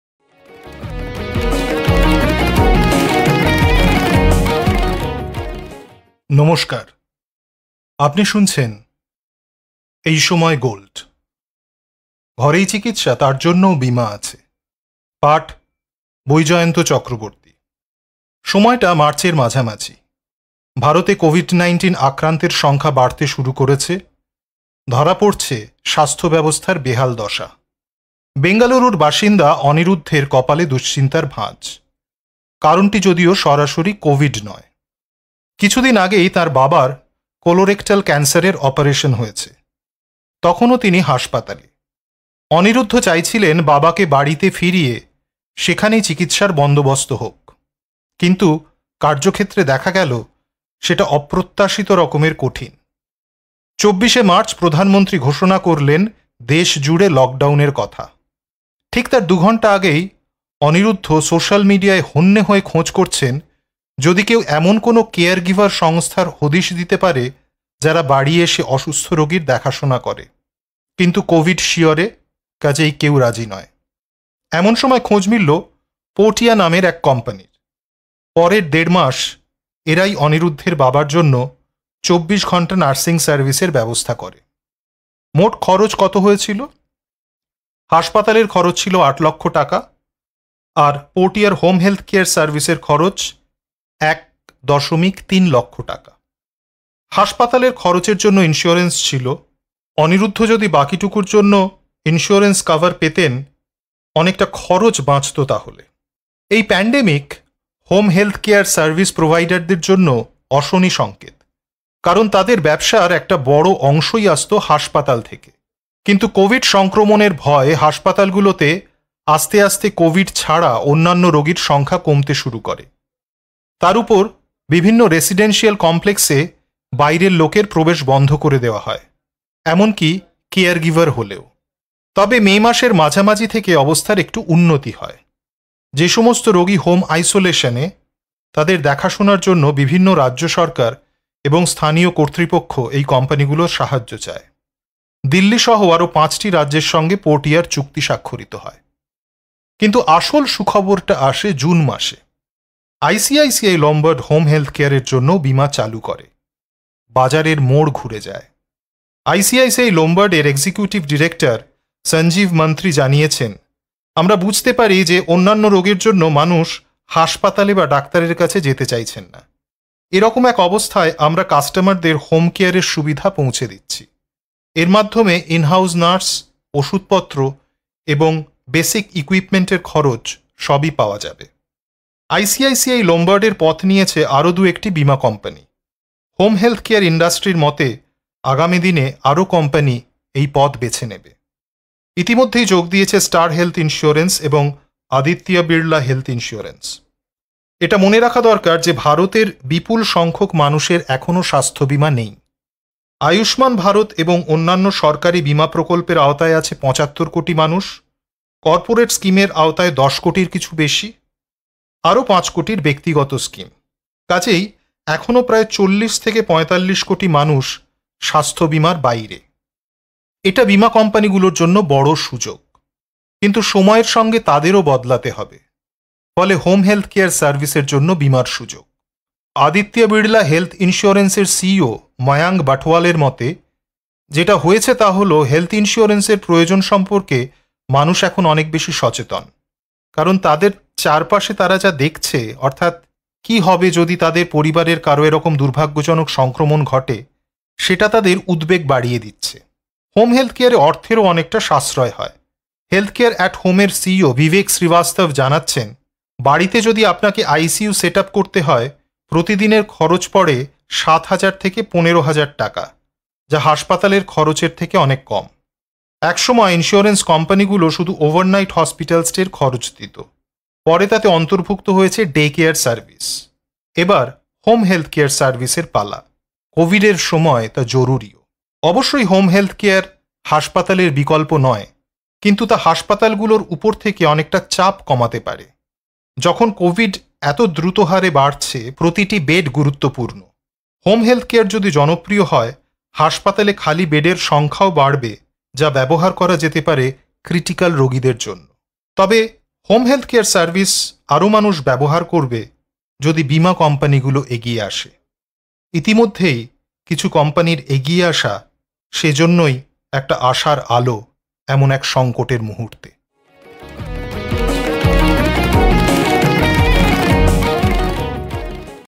ঘরেই চিকিৎসা? তার জন্যও বিমা আছে
সম্প্রতি আইসিসিআই লোম্বার্ড হোম হেলথকেয়ারের জন্যও বিমা চালু করে। বাজারের মোড় ঘুরে যায়। আইসিসিআই লোম্বার্ড-এর পথ নিয়েছে আরও দু’একটি বিমা কোম্পানি।

ET প্রাইম

সময়টা মার্চের মাঝামাঝি। ভারতে কোভিড-১৯ আক্রান্তের সংখ্যা বাড়তে শুরু করেছে। ধরা পড়ছে স্বাস্থ্যব্যবস্থার বেহাল দশা। বেঙ্গালুরুর বাসিন্দা অনিরুদ্ধের কপালে দুশ্চিন্তার ভাঁজ। কারণটি যদিও সরাসরি কোভিড নয়।

কিছু দিন আগেই তাঁর বাবার কোলোরেক্টাল ক্যান্সারের অপারেশন হয়েছে। তখনও তিনি হাসপাতালে। অনিরুদ্ধ চাইছিলেন বাবাকে বাড়িতে ফিরিয়ে সেখানেই চিকিৎসার বন্দোবস্ত হোক। কিন্তু কার্যক্ষেত্রে দেখা গেল, সেটা অপ্রত্যাশিত রকমের কঠিন।

২৪ মার্চ প্রধানমন্ত্রী ঘোষণা করলেন দেশজুড়ে লকডাউনের কথা। ঠিক তার দু’ঘণ্টা আগেই অনিরুদ্ধ সোশ্যাল মিডিয়ায় হন্যে হয়ে খোঁজ করছেন, যদি কেউ এমন কোনও কেয়ারগিভার সংস্থার হদিশ দিতে পারে, যারা বাড়ি এসে অসুস্থ রোগীর দেখাশোনা করে। কিন্তু কোভিড শিয়রে, কাজেই কেউ রাজি নয়।
এমন সময় খোঁজ মিলল পোর্টিয়া নামের এক কোম্পানির। পরের দেড় মাস এরাই অনিরুদ্ধের বাবার জন্য ২৪ ঘণ্টা নার্সিং সার্ভিসের ব্যবস্থা করে।

মোট খরচ কত হয়েছিল? হাসপাতালের খরচ ছিল ৮ লক্ষ টাকা, আর পোর্টিয়া-র হোম-হেলথকেয়ার সার্ভিসের খরচ ১.৩ লক্ষ টাকা। হাসপাতালের খরচের জন্য ইনস্যুরেন্স ছিল। অনিরুদ্ধ যদি বাকিটুকুর জন্য ইনস্যুরেন্স কভার পেতেন, অনেকটা খরচ বাঁচত তা হলে।

এই প্যানডেমিক হোম-হেলথকেয়ার সার্ভিস প্রোভাইডারদের জন্যও অশনি সংকেত। কারণ তাদের ব্যবসার একটা বড় অংশই আসত হাসপাতাল থেকে। কিন্তু কোভিড সংক্রমণের ভয়ে হাসপাতালগুলোতে আস্তে আস্তে কোভিড ছাড়া অন্যান্য রোগীর সংখ্যা কমতে শুরু করে। তার উপর বিভিন্ন রেসিডেনশিয়াল কমপ্লেক্সে বাইরের লোকের প্রবেশ বন্ধ করে দেওয়া হয়, এমনকী কেয়ারগিভার হলেও।

তবে মে মাসের মাঝামাঝি থেকে অবস্থার একটু উন্নতি হয়। যে সমস্ত রোগী হোম আইসোলেশনে, তাদের দেখাশোনার জন্য বিভিন্ন রাজ্য সরকার এবং স্থানীয় কর্তৃপক্ষ এই কোম্পানিগুলোর সাহায্য চায়। দিল্লি-সহ আরও পঁচটি রাজ্যের সঙ্গে পোর্টিয়ার চুক্তি স্বাক্ষরিত হয়।

কিন্তু আসল সুখবরটা আসে জুন মাসে। আইসিসিআই লোম্বার্ড হোম হেলথকেয়ারের জন্যও বিমা চালু করে। বাজারের মোড় ঘুরে যায়। আইসিসিআই লোম্বার্ড-এর এক্সিকিউটিভ ডিরেক্টর সঞ্জীব মন্ত্রী জানিয়েছেন, ‘আমরা বুঝতে পারি যে অন্যান্য রোগের জন্য মানুষ হাসপাতালে বা ডাক্তারের কাছে যেতে চাইছেন না। এ রকম এক অবস্থায় আমরা কাস্টমারদের হোম কেয়ারের সুবিধা পৌঁছে দিচ্ছি। এর মাধ্যমে ইন-হাউস নার্স, ওষুধপত্র এবং বেসিক ইকুইপমেন্টের খরচ, সবই পাওয়া যাবে।’ আইসিসিআই লোম্বার্ড-এর পথ নিয়েছে আরও দু’একটি বিমা কোম্পানি। হোম-হেলথকেয়ার ইন্ডাস্ট্রির মতে, আগামী দিনে আরও কোম্পনি এই পথ বেছে নেবে।

ইতিমধ্যেই যোগ দিয়েছে স্টার হেলথ ইনস্যুরেন্স এবং আদিত্য বিড়লা হেলথ ইনস্যুরেন্স। এটা মনে রাখা দরকার যে ভারতের বিপুল সংখ্যক মানুষের এখনও স্বাস্থ্যবিমা নেই। আয়ুষ্মান ভারত এবং অন্যান্য সরকারি বিমা প্রকল্পের আওতায় আছে ৭৫ কোটি মানুষ, কর্পোরেট স্কিমের আওতায় ১০ কোটির কিছু বেশি, আরও ৫ কোটির ব্যক্তিগত স্কিম। কাজেই এখনও প্রায় ৪০-৪৫ কোটি মানুষ স্বাস্থ্যবিমার বাইরে। এটা বিমা কোম্পানিগুলোর জন্য বড় সুযোগ। কিন্তু সময়ের সঙ্গে তাদেরও বদলাতে হবে। ফলে হোম-হেলথকেয়ার সার্ভিসের জন্য বিমার সুযোগ।

আদিত্য বিড়লা হেলথ ইনস্যুরেন্স-এর সিইও ময়াঙ্ক বাঠোয়ালের মতে, ‘যেটা হয়েছে তা হল হেলথ ইন্স্যুরেন্সের প্রয়োজন সম্পর্কে মানুষ এখন অনেক বেশি সচেতন। কারণ তাদের চারপাশে তারা যে দেখছে, অর্থাৎ কী হবে যদি তাদের পরিবারের কারও এ রকম দুর্ভাগ্যজনক সংক্রমণ ঘটে, সেটা তাদের উদ্বেগ বাড়িয়ে দিচ্ছে।’

হোম হেলথকেয়ারে অর্থেরও অনেকটা সাশ্রয় হয়। হেলথকেয়ার অ্যাট হোম-এর সিইও বিবেক শ্রীবাস্তব জানাচ্ছেন, বাড়িতে যদি আপনাকে আইসিইউ সেট-আপ করতে হয়, প্রতি দিনের খরচ পড়ে ৭ হাজার থেকে ১৫ হাজার টাকা, যা হাসপাতালের খরচের থেকে অনেক কম।

এক সময় ইনস্যুরেন্স কোম্পানিগুলো শুধু ওভারনাইট হসপিটাল স্টে-র খরচ দিত। পরে তাতে অন্তর্ভুক্ত হয়েছে ডে কেয়ার সার্ভিস। এবার হোম হেলথকেয়ার সার্ভিসের পালা। কোভিডের সময় তা জরুরিও।

অবশ্যই হোম হেলথকেয়ার হাসপাতালের বিকল্প নয়, কিন্তু তা হাসপাতালগুলোর উপর থেকে অনেকটা চাপ কমাতে পারে। যখন কোভিড এত দ্রুত হারে বাড়ছে, প্রতিটি বেড গুরুত্বপূর্ণ। হোম হেলথকেয়ার যদি জনপ্রিয় হয়, হাসপাতালে খালি বেডের সংখ্যাও বাড়বে, যা ব্যবহার করা যেতে পারে ক্রিটিকাল রোগীদের জন্য।

তবে হোম হেলথকেয়ার সার্ভিস আরও মানুষ ব্যবহার করবে, যদি বিমা কোম্পানিগুলো এগিয়ে আসে। ইতিমধ্যেই কিছু কোম্পানির এগিয়ে আসা সে জন্যই একটা আশার আলো এমন এক সংকটের মুহূর্তে।